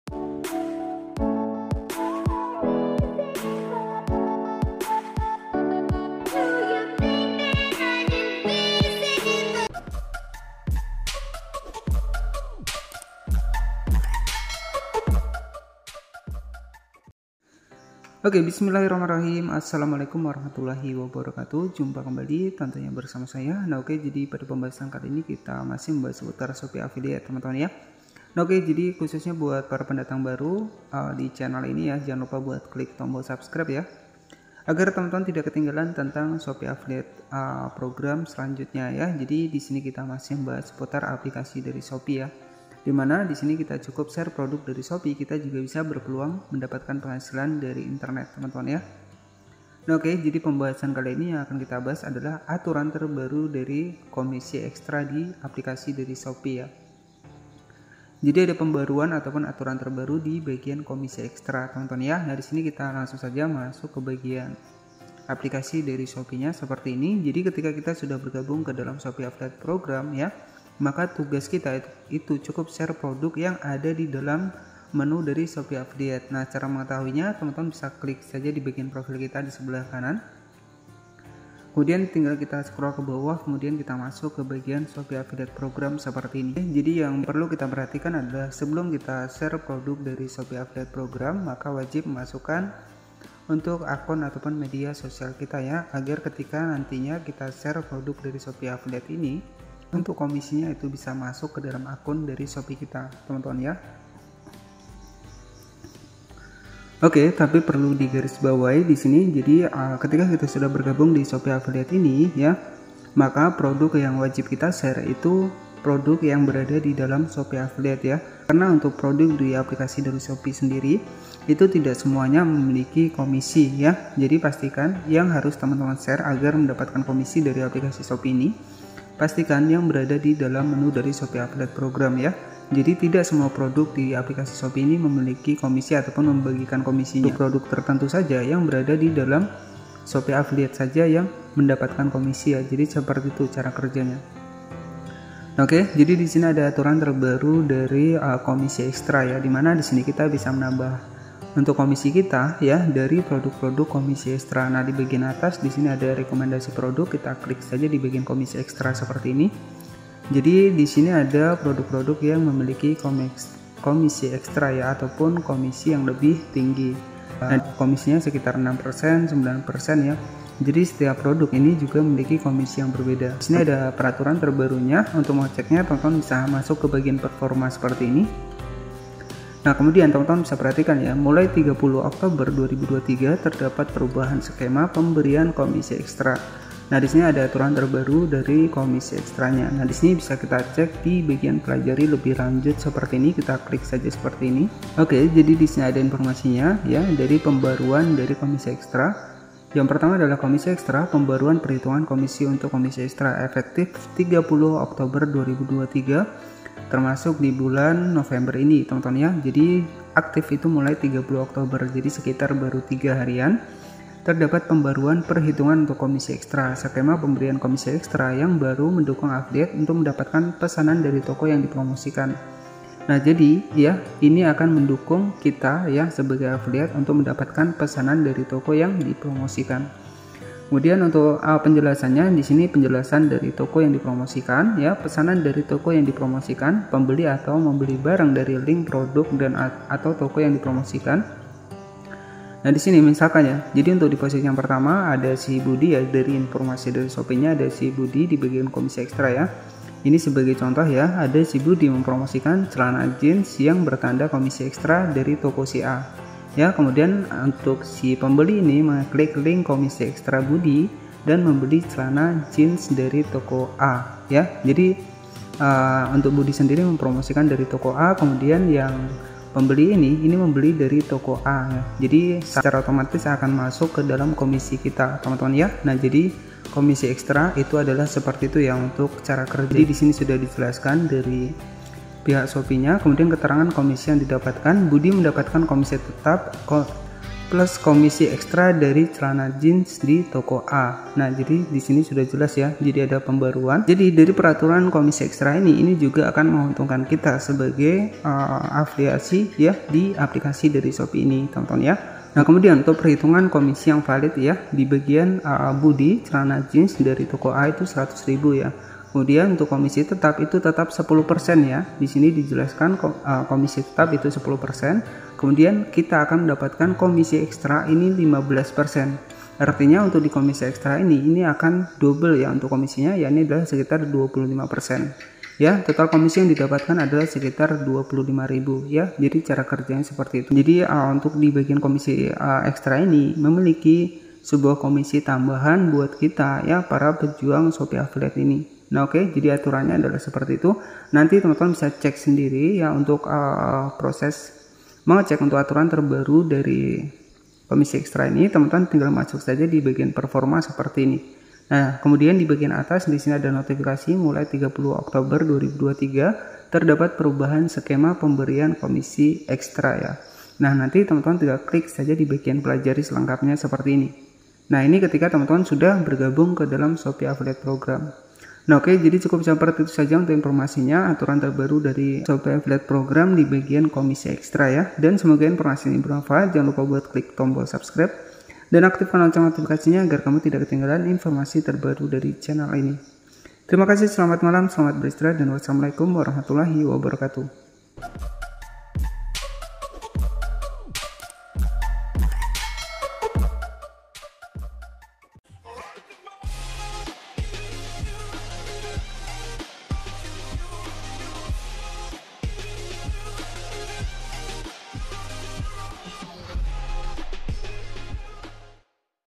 Aturan Baru Shopee Affiliate Program di Bagian Komisi Xtra Pemula Wajib Tau ini
hallo gyu's selamat datang kembali di channel saya, oke di pembahasan kali ini saya akan membahas, Aturan Baru Shopee Affiliate Program di Bagian Komisi Xtra Pemula Wajib Tau ini, semoga bermanfaat dan jangan lupa subscribe,like, share ok, terimakasih